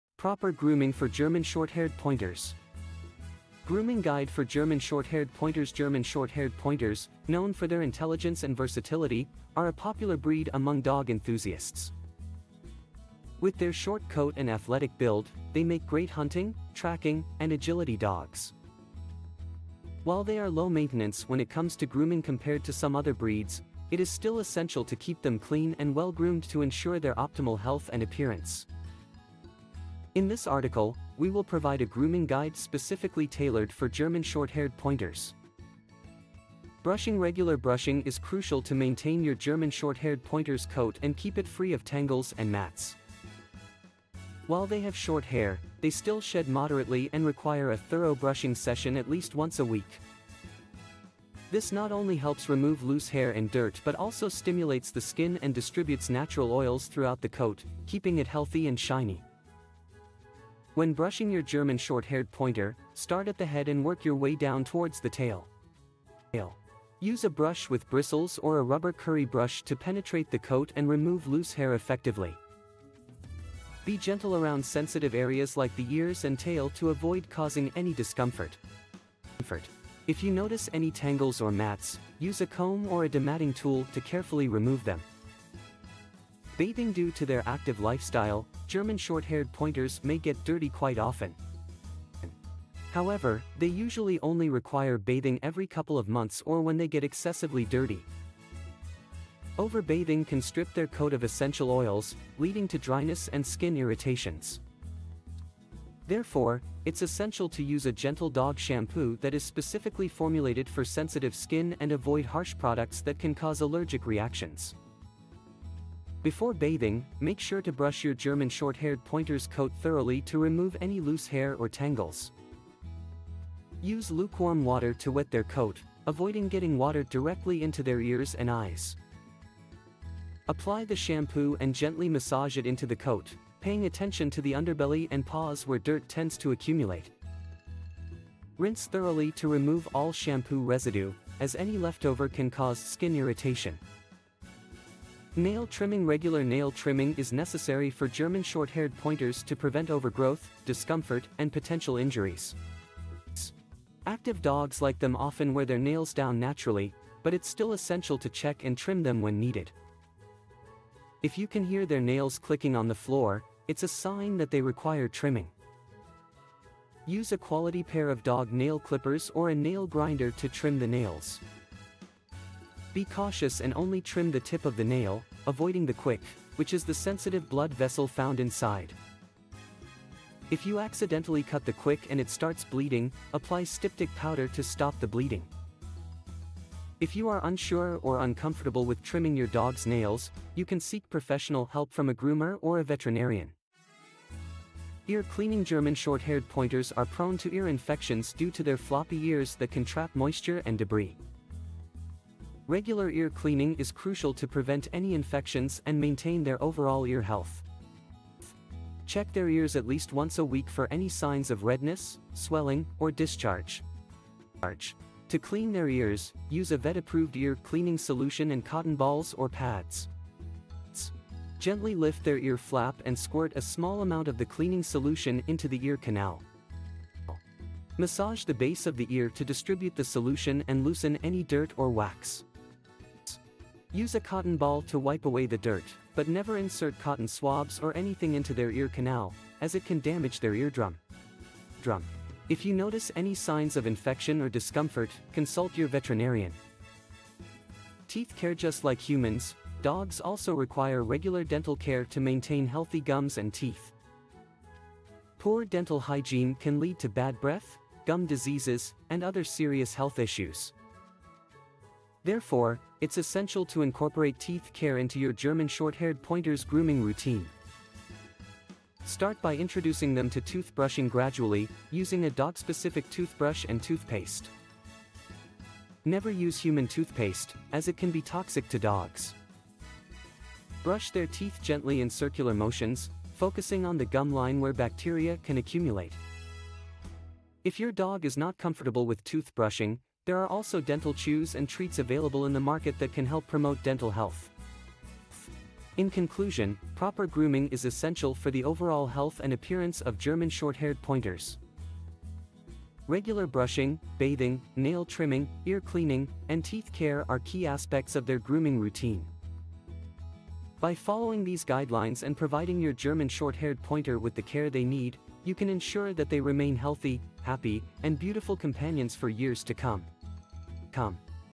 Proper Grooming for German Shorthaired Pointers
Grooming Guide for German Shorthaired Pointers

German Shorthaired Pointers, known for their intelligence and versatility, are a popular breed among dog enthusiasts  With their short coat and athletic build, they make great hunting, tracking, and agility dogs  While they are low-maintenance when it comes to grooming compared to some other breeds, it is still essential to keep them clean and well-groomed to ensure their optimal health and appearance  In this article, we will provide a grooming guide specifically tailored for German Shorthaired Pointers

Brushing

Regular brushing is crucial to maintain your German Shorthaired Pointer's coat and keep it free of tangles and mats
00:00 Proper Grooming for German Shorthaired Pointers